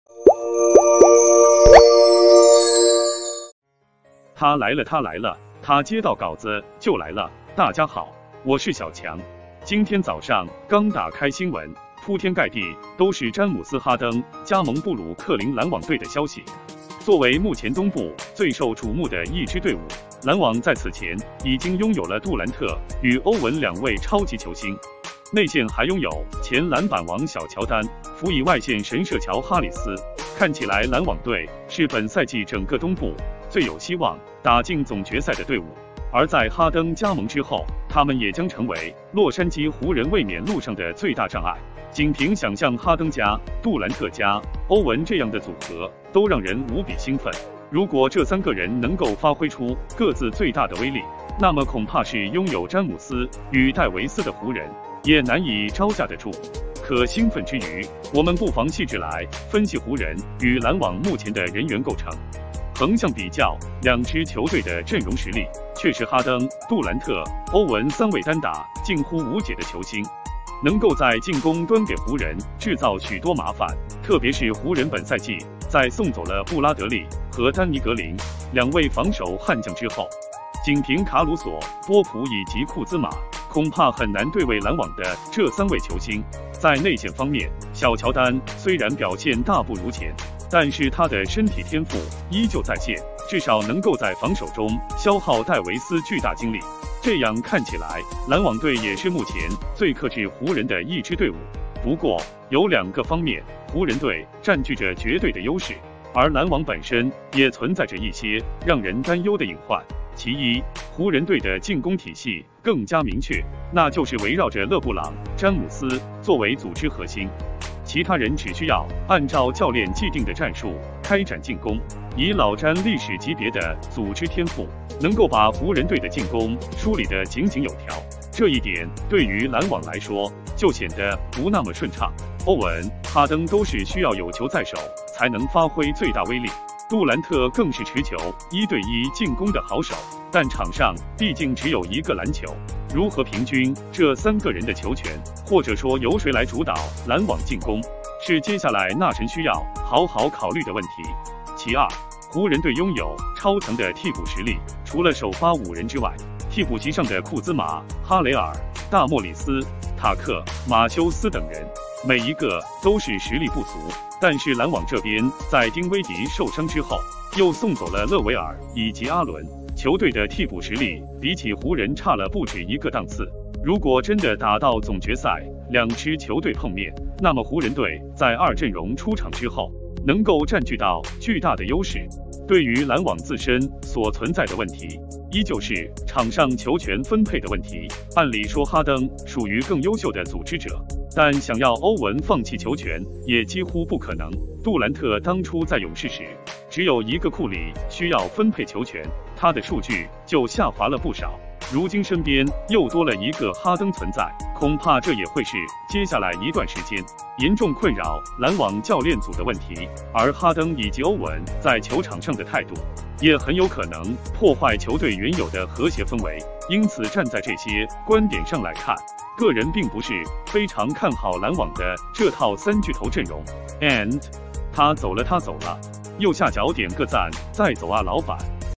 篮网三巨头不怕！横向比较“湖人”两个细节更占优势，一人陷入最大隐患
今天早上刚打开新闻，铺天盖地都是詹姆斯-哈登加盟布鲁克林篮网队的消息，作为目前东部最受瞩目的一支队伍，篮网在此前已经拥有了杜兰特与欧文两位超级球星，内线还拥有前篮板王小乔丹，辅以外线神射乔-哈里斯，看起来篮网队是本赛季整个东部最有希望打进总决赛的队伍，而在哈登加盟之后，他们也将成